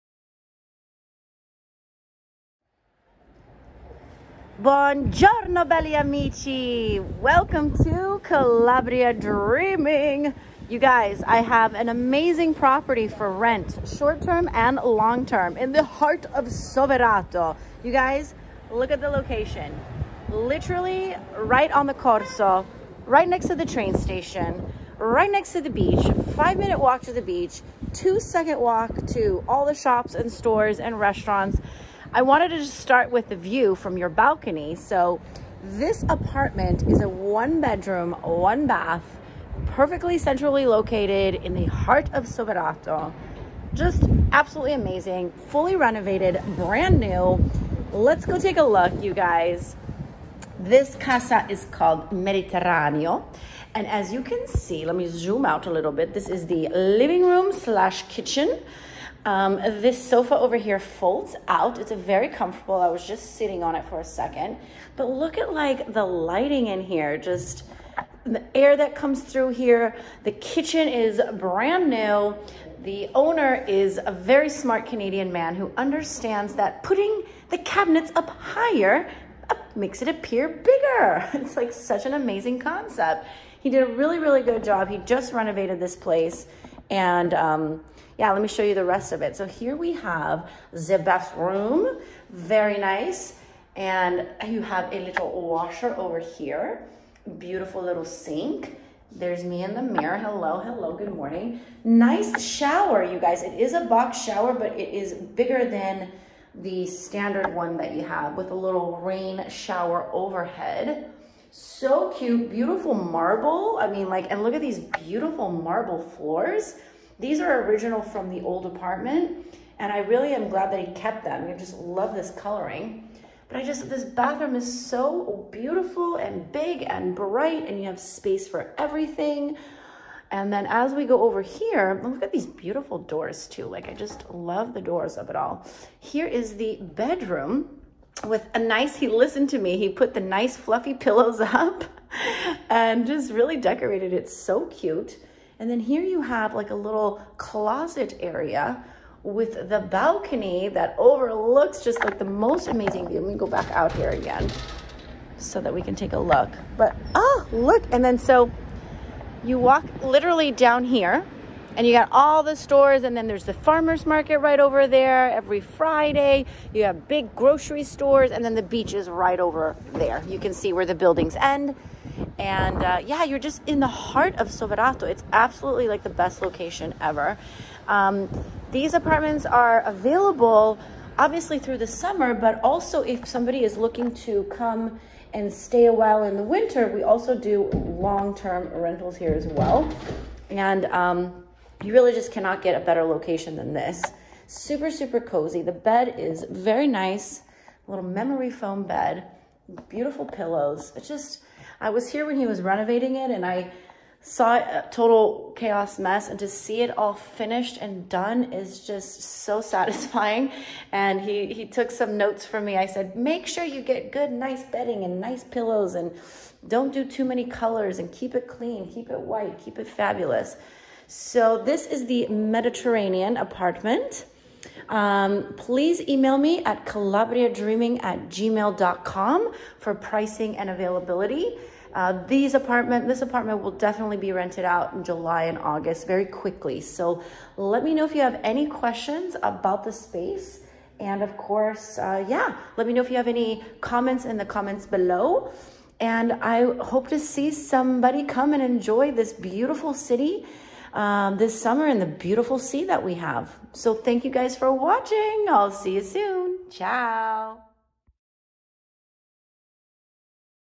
FABULOUS RENTAL IN HEART OF SOVERATO
You guys!!! If you are thinking of coming to Calabria on vacation look no further!! I have a fantastic apartment for short and long term rent in the heart of the best city Calabria has to offer! This one bedroom, one bath apartment is right on the main drag, only two mins away from all the shopping and restaurants, 30 seconds away from the train station and only 5 mins walking from the best beaches. You are far away from all the crazy discos in the summer and high up enough to enjoy the cool breeze from the mountains. I hope to see you soon!